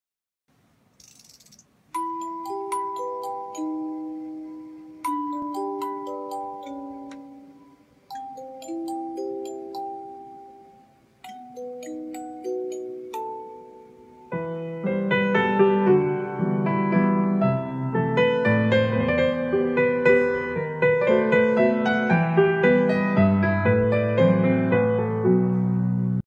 How to play Music Box and Classical piano
This is an arrangement of rock Japanese style combined with classical style

I am the arranger of this song PhebeChou
這是一首日系搖滾風結合古典風格的編曲
我是這首歌的編曲 周菲比

如果你想要知道更多的“編曲家的日常”你可以追蹤我